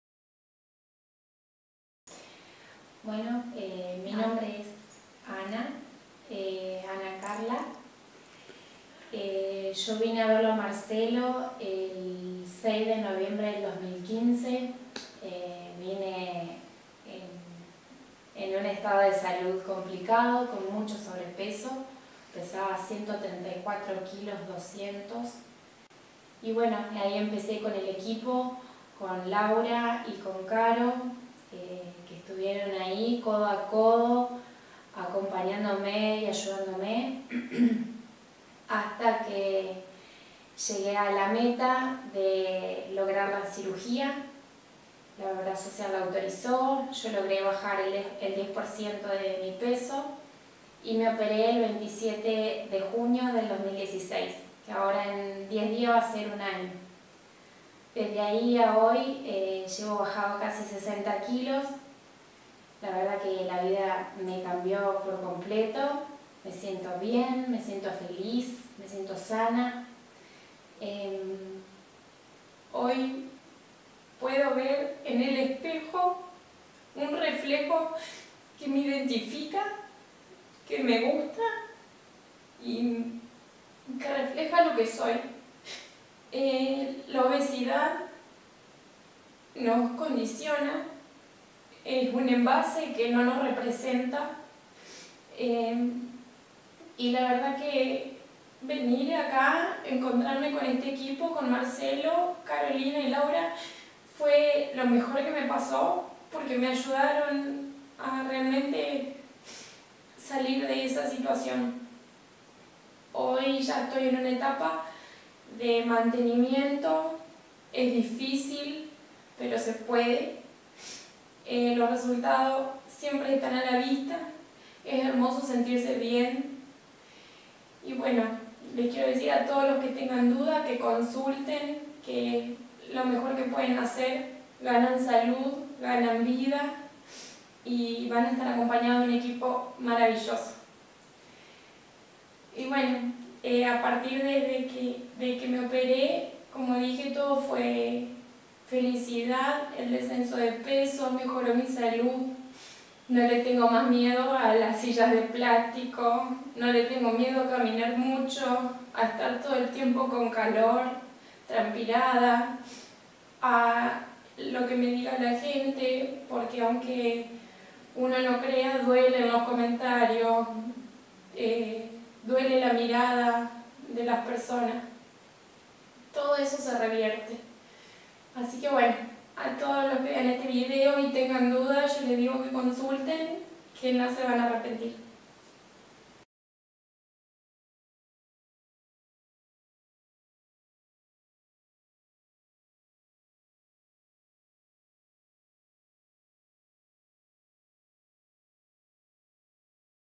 Testimonio de la paciente Ana Carla Yauck
Ana Carla: "Hoy puedo ver en el espejo un reflejo que me identifica, que me gusta y que refleja lo que soy. La obesidad nos condiciona, es un envase que no nos representa."